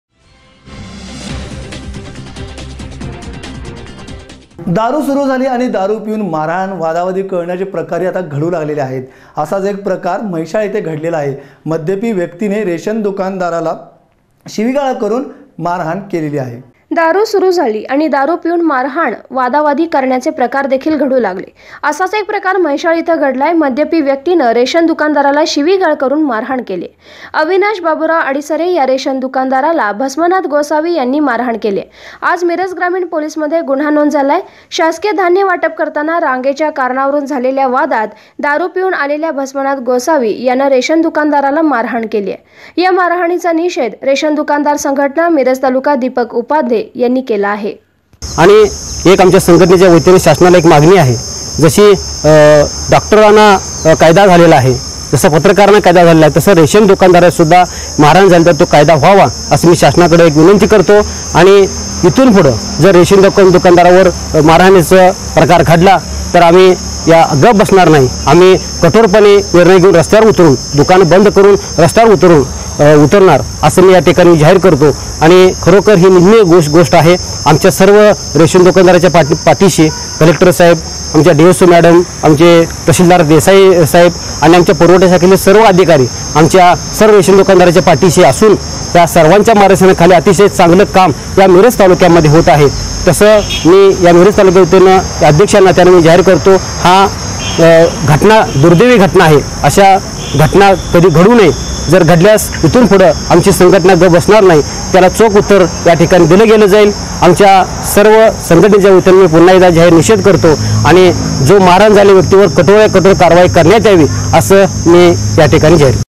दारू दुकाने सुरु झाल्यापासून मारहाणीच्या प्रकारात वाढ
दारू दुकाने सुरु झाल्यापासून मारहाणीच्या प्रकारात वाढ, मिरज म्हैशाळ येथे दारू पिलेल्या व्यक्तीने रेशन दुकानदाराला केली मारहाण.@